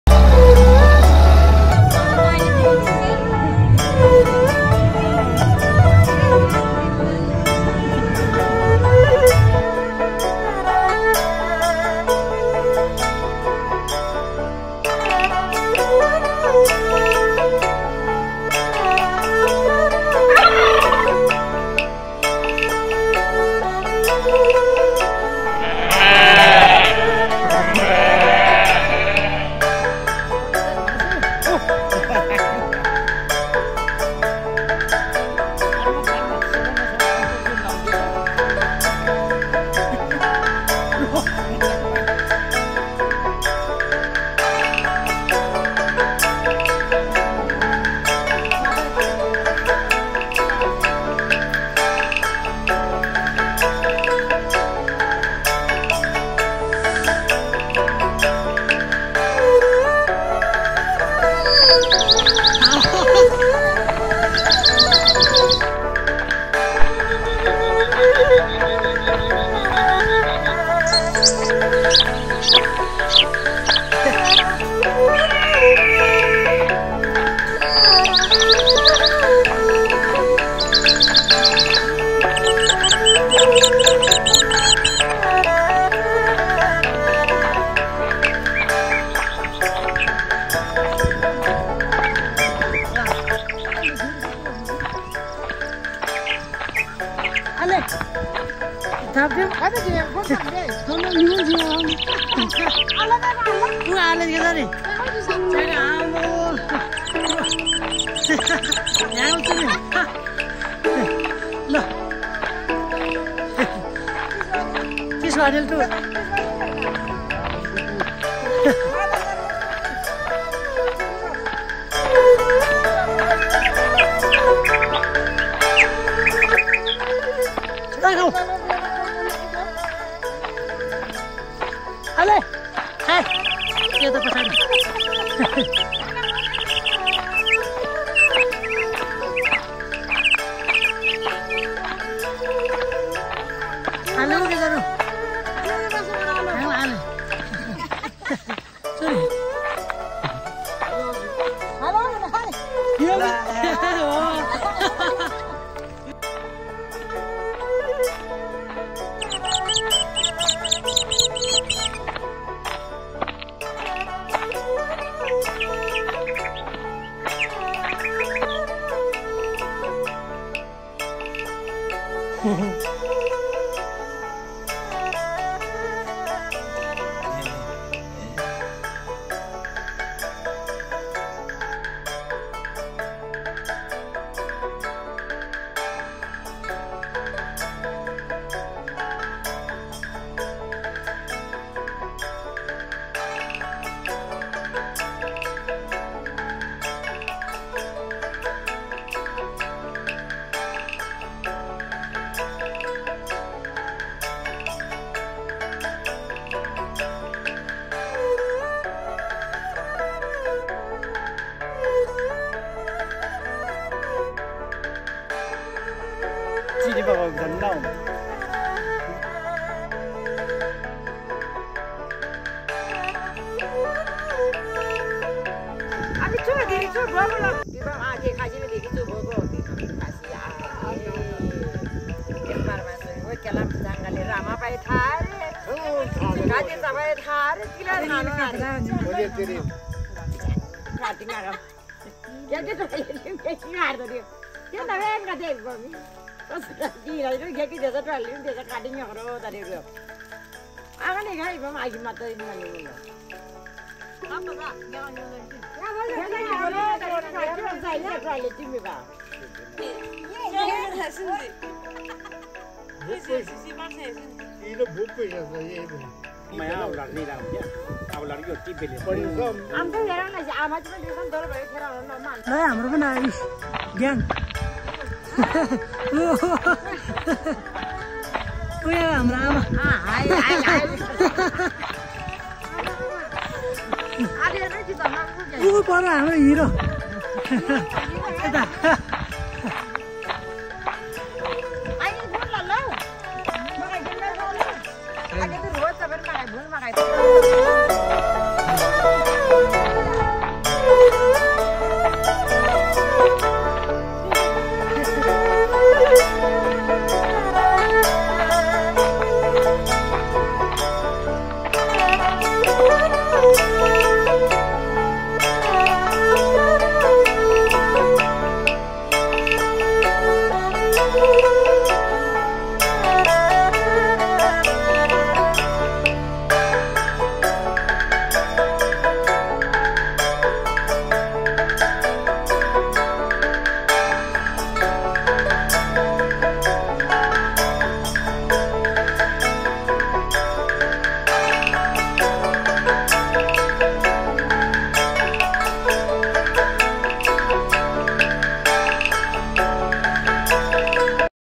Peaceful and Natural Views of my Village 🇳🇵 🇳🇵 🇳🇵🙏🙏🙏
Village vlog
Having lot’s of fun in village